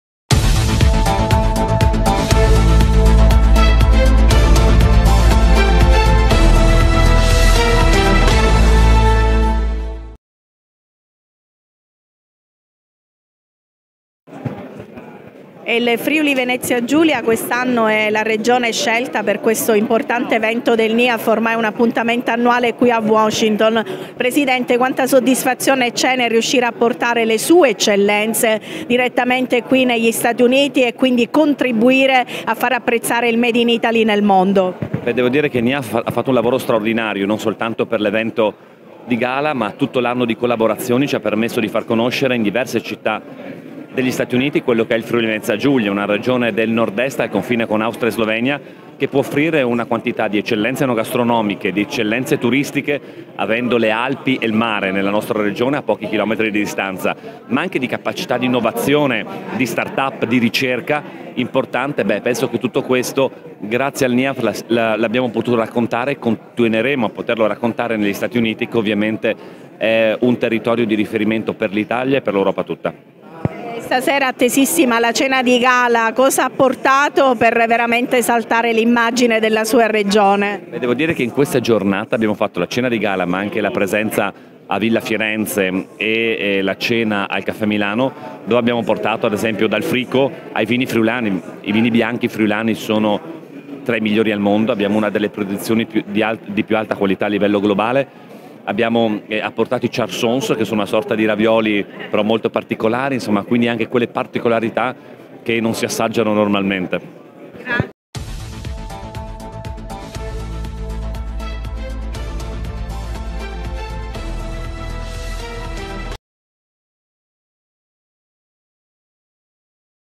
Intervista al Presidente del Friuli Venezia Giulia Dott. Massimiliano Fedriga
Italian's News intervista il Presidente della Regione Friuli Venezia Giulia Dott. Massimiliano Fedriga al Gala del NIAF - National Italian American Foundation, Washington DC.
Il Presidente Fedriga è stato premiato al 45° incontro NIAF.
A cura dell'inviata Marilena Alescio.
Montaggio: Giovanni Maria Ferrara.